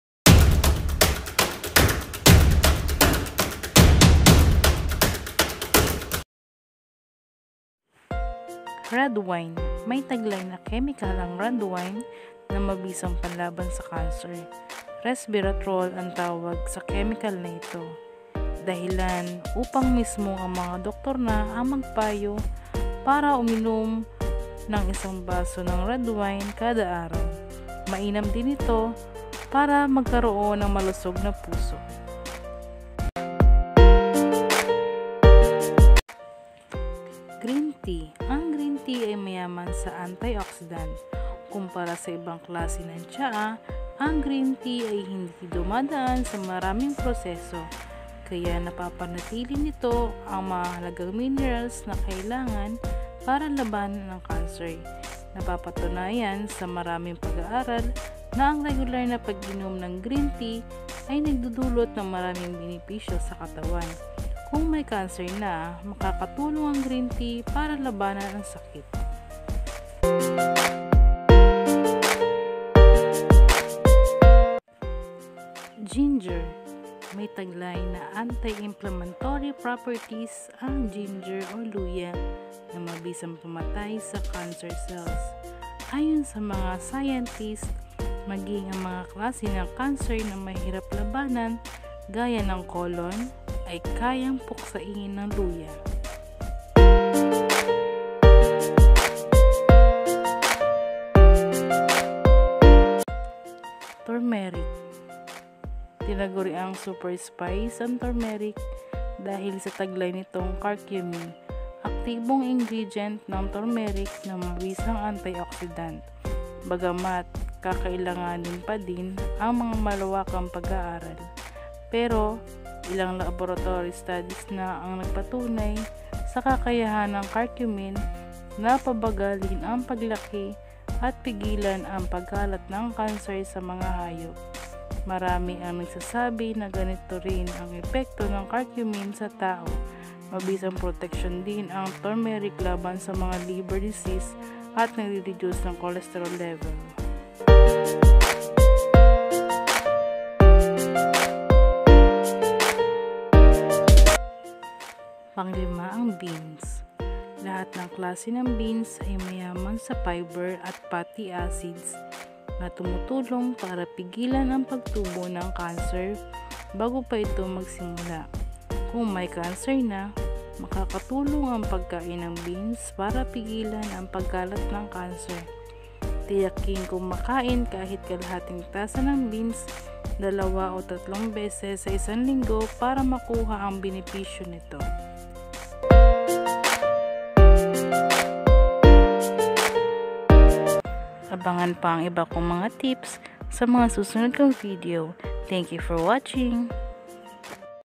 5 TIPS ANTI-CANCER FOOD AND DRINKS
1. RED WINE - May taglay na kemikal ang red wine na mabisang panlaban sa cancer. Resveratrol ang tawag sa kemikal na ito. Ito ang dahilan kaya mismong mga doctor ang ipinapayo ang pag inom ng isang baso ng red wine kada araw. Mainam din ang red wine para magkaroon ng malusog na puso.

2. GREEN TEA - Ang green tea ay mayaman sa anti-oxidants. Kumpara sa ibang klase ng tsaa ang green tea ay hindi dumaan sa maraming proseso kaya napapanatili niti ang mahahalagang minerals na kailangan para labanan ang cancer. Napatunayan sa maraming pag-aaral na ang regular na pag-inom ng green tea ay nagdudulot ng maraming benepisyo sa katawan. Kung may cancer na, makakatulong ang green tea para labanan ang sakit.

3. GINGER - May taglay na anti-inflammatory properties ang ginger o luya na mabisang pamatay sa cancer cells. Ayon sa mga scientiest, maging ang mga klase ng cancer na mahirap labanan gaya ng colon ay kayang puksain ng luya.

4. TURMERIC - Tinaguriang super spice ang turmeric dahil sa taglay nitong "curcumin", aktibong ingredient ng turmeric na mabisang anti-oxidant. Bagamat kakailanganin pa din ang malawakang pag-aaral, pero ilang laboratory studies na ang nagpatunay sa kakayahan ng curcumin na pabagalin ang paglaki at pigilan ang pagkalat ng cancer sa mga hayop. Maramai ang nagsasabi na ganito rin ang epekto ng curcumin sa tao. Mabisang proteksyon din ang turmeric laban sa mga liver diseases at nagre-reduce ng cholesterol levels.

5. BEANS - Lahat ng klase ng beans ay mayaman sa fiber at fatty acids na tumutulong para pigilan ang pagtubo ng cancer bago pa ito magsimula. Kung may cancer na, makakatulong ang pagkain ng beans para pigilan ang pagkalat ng cancer. Tiyaking kumain kahit kalahating tasa ng beans dalawa o tatlong beses sa isang linggo para makuha ang benepisyo nito.